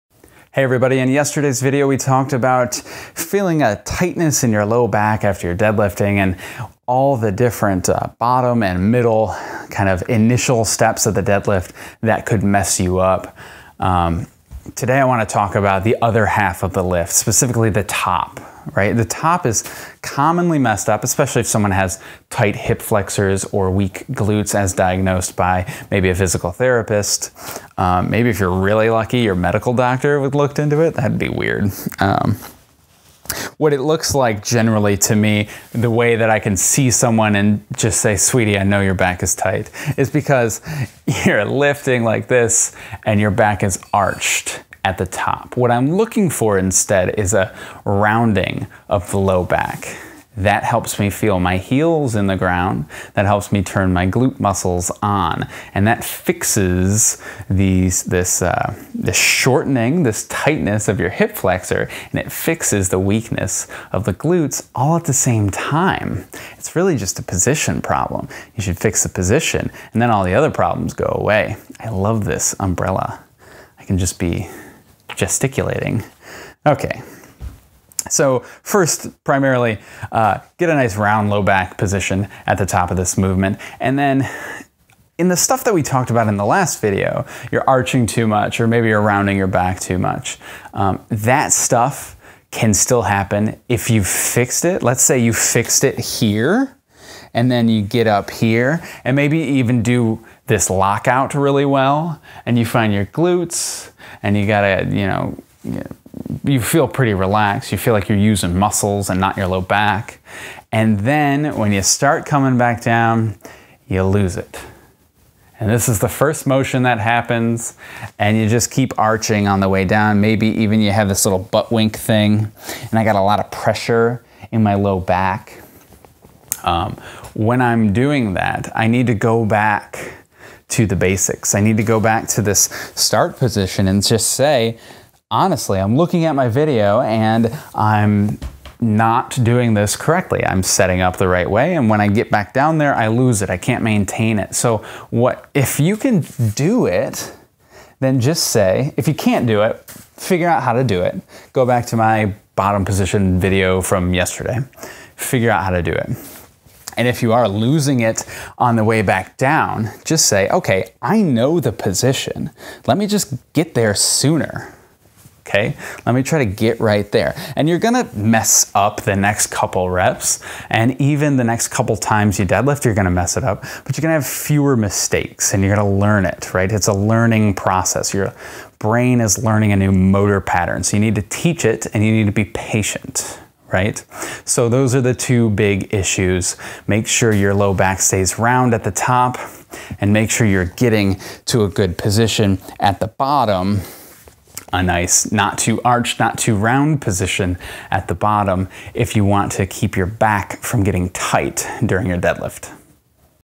Tight Low Back After Deadlifting
Get those glutes if you want to decompress your low back.